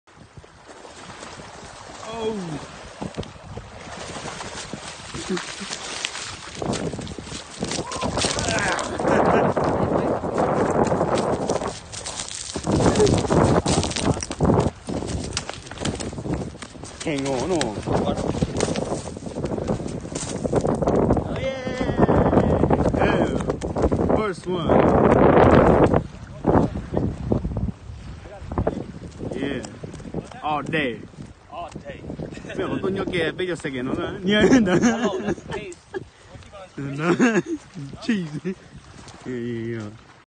Snagging King Salmon
here a clip of my brother in law skinny landing a king salmon at seward 2020 video capture by my bro in law robert it